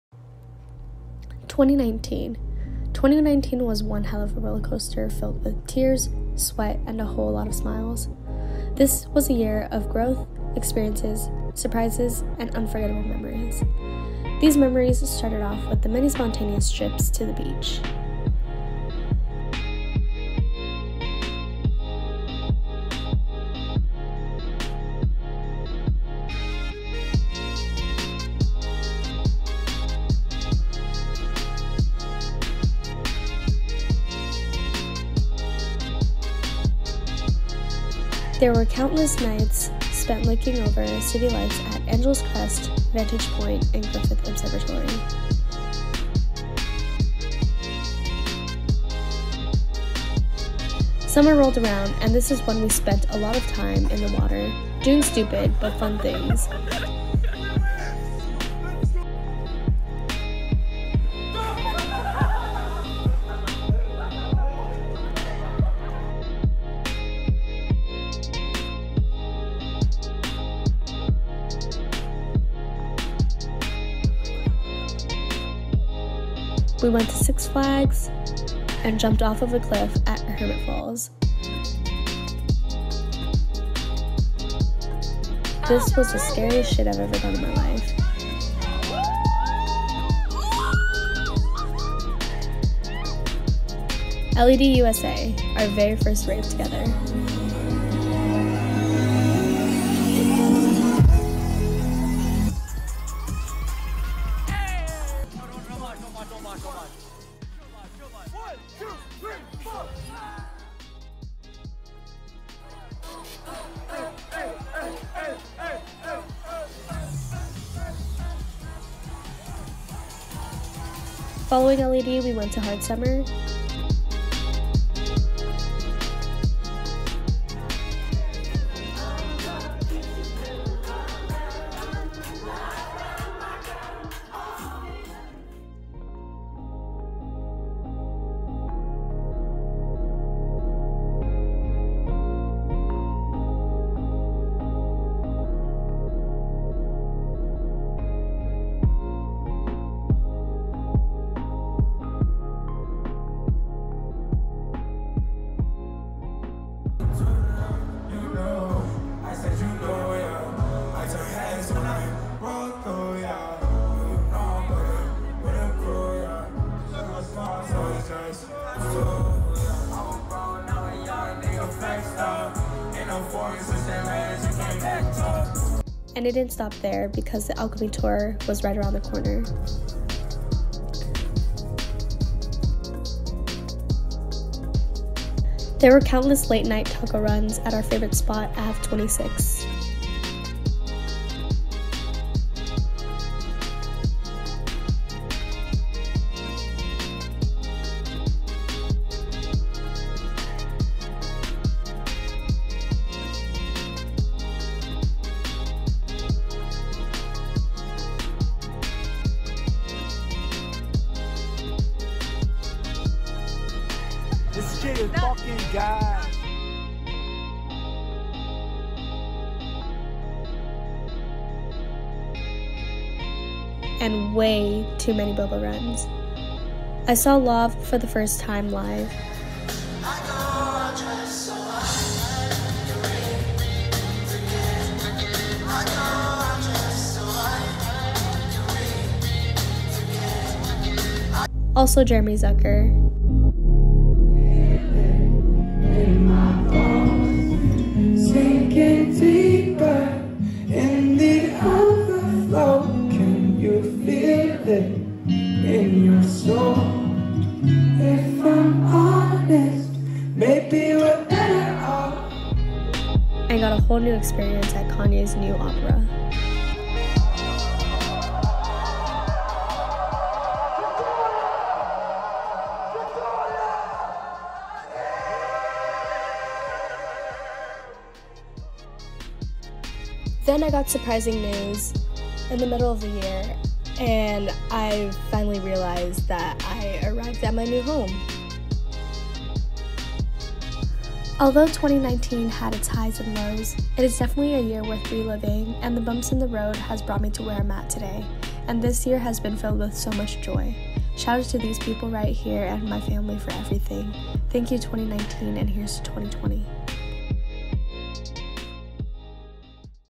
2019 RECAP
wrapping up the end of a decade. 2019 was an unforgettable year. thank you to everyone in my life who made it so fun. i lived up to every moment this year, and i am so glad i got to experience it with the people who never let me down.